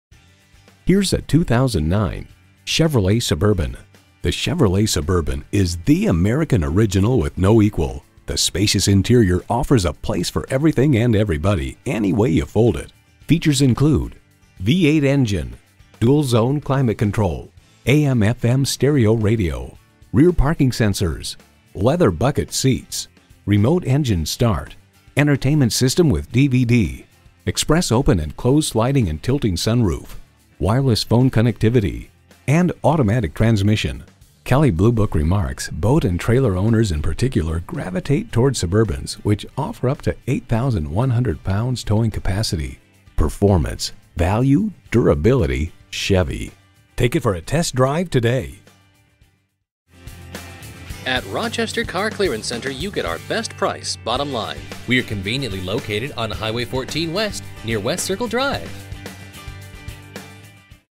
2009 Chevrolet Suburban 1500 Rochester Winona, MN FB218084 - SOLD
Year: 2009
Make: Chevrolet
Model: Suburban 1500
Trim: LT
Engine: Vortec 5.3L V8 SFI Flex Fuel
Transmission: 6-Speed Automatic Electronic with Overdrive
Color: Gray
Interior: Light Cashmere/Dark Cashmere
Mileage: 195201
Stock #: FB218084
VIN: 1GNFK26369R234046
Recent Arrival! We look forward to earning your business!

Tune-Ups
Transmission Service
Brake Service
Performance Parts
Factory Wheels

Address:
2980 HWY 14